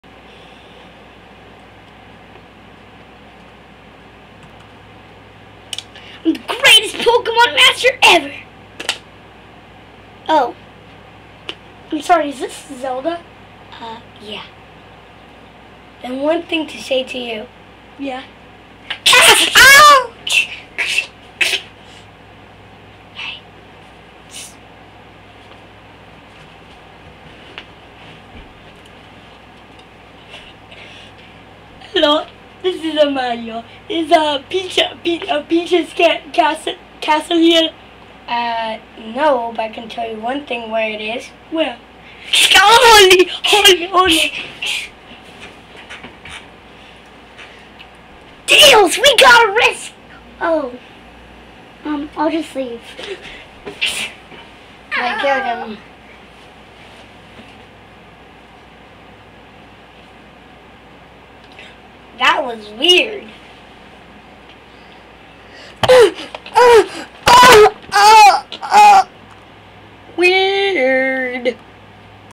where am I?
link just stands there while difrent characters pop out